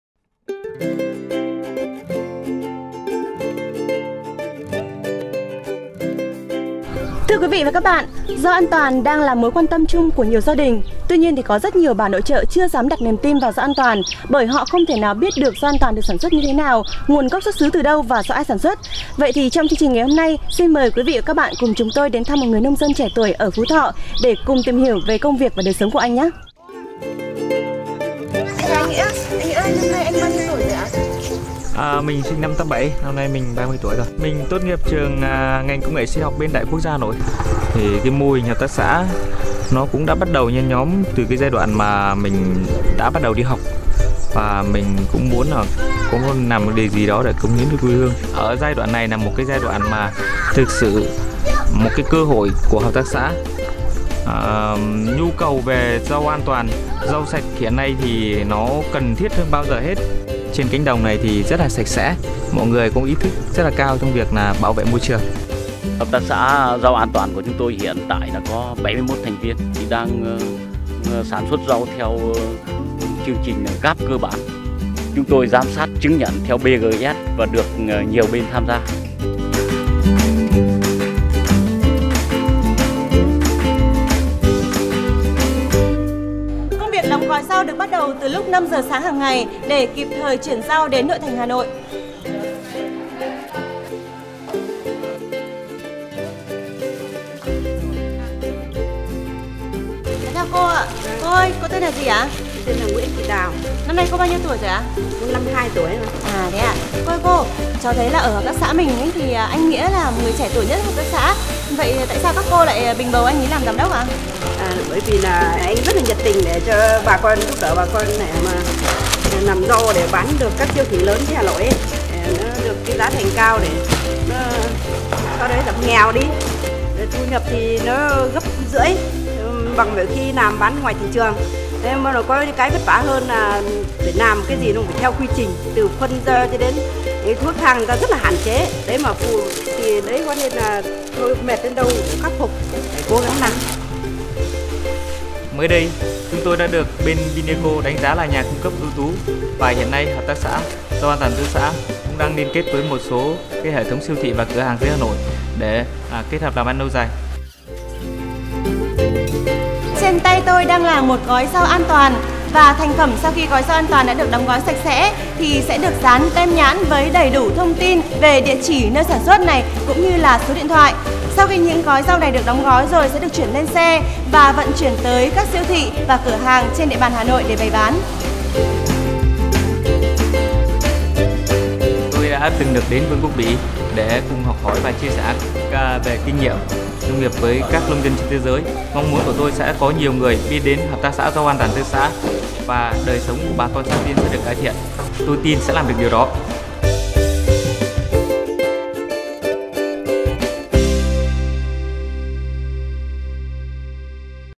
PGS in practice - Meet Nguyen Van Nghia, leader of Tu Xa Cooperative
This video draws the portrait of Mr. Nguyen Van Nghia, the leader of Tu Xa Cooperative in Phu Tho province. Together with VECO/Rikolto and the provincial quality assurance department, Nghia has spearheaded the development of a Participatory Guarantee System (PGS) in his cooperative. Thanks to PGS, members of the cooperative are able to guarantee the quality of their BasicGAP-compliant vegetables at a low cost. This new certifications opened the door to new ventures and helped the cooperative access urban safe vegetables market. Tu Xa is now supplying its vegetables to one of the biggest minimart chain in Vietnam.